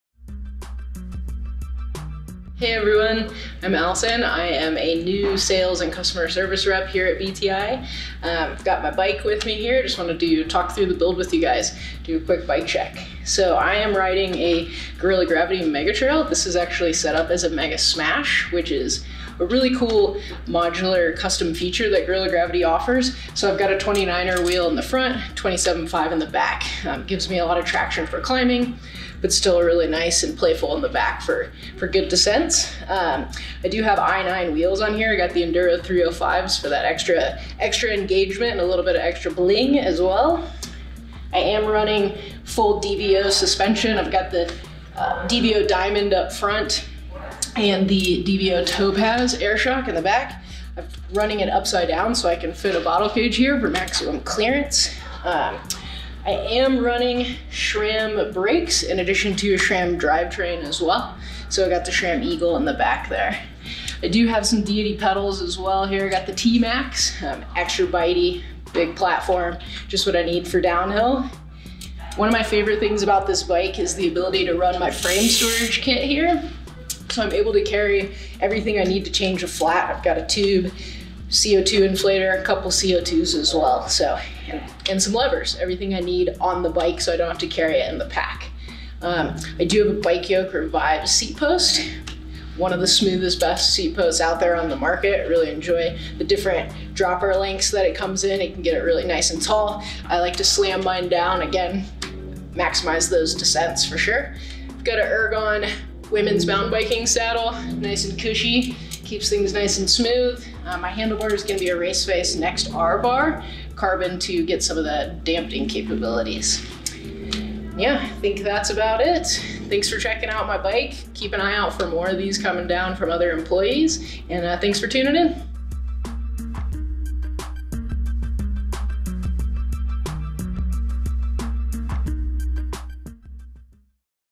Bike Check Bonanza // Guerrilla Gravity Megatrail // Megasmash
Today's Bike Check Bonanza is with Allison, a new Sales and Customer Service Associate here at BTI. Join us as she breaks down her killer Guerrilla Gravity Megatrail which has been set up as a Megasmash!

With a 29” wheel in the front and 27.5” wheel in the back this set up allows for great traction in the front for climbing as well as retaining its playfulness in the back for descents.

Parts:
I-Nine Wheels - Enduro 305

Sram Brakes and Drive Train